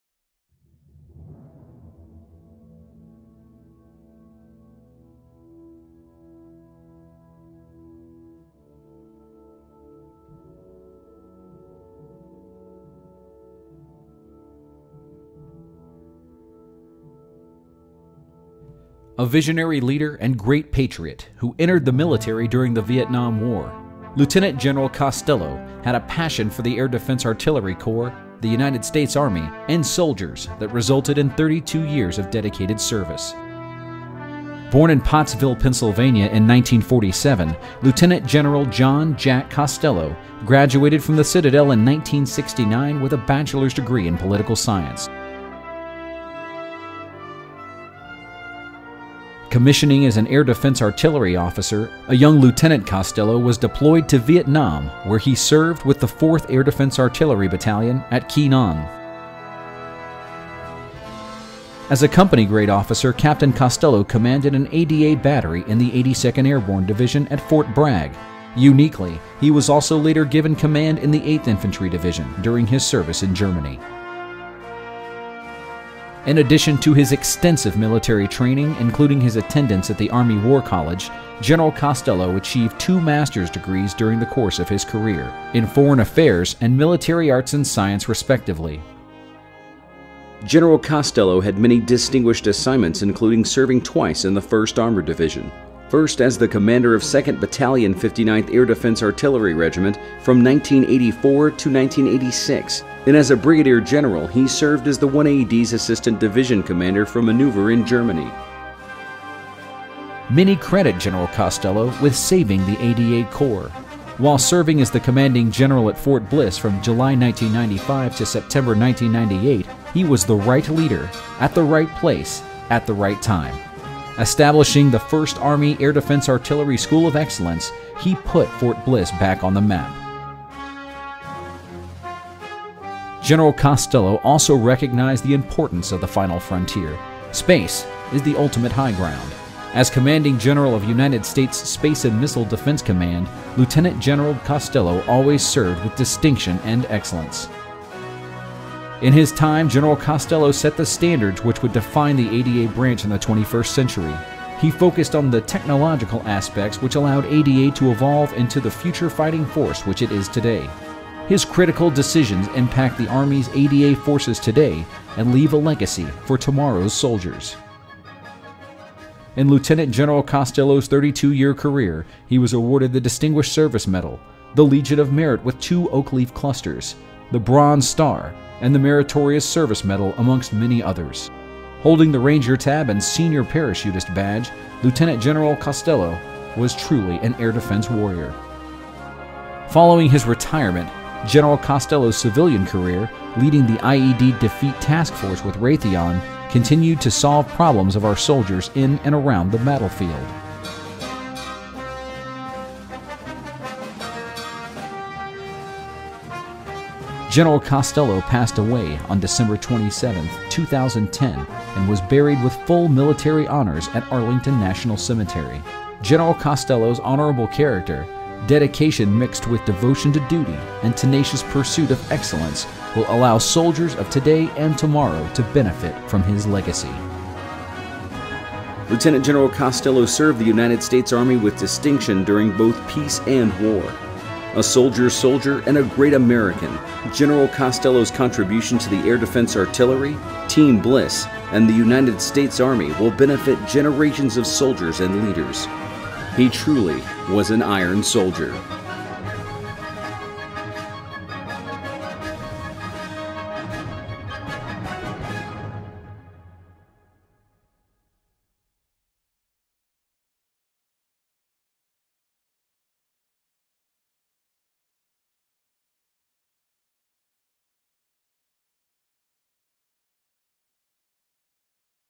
LTG John Costello Dedication Video
Short video about the life and career of LTG John Costello.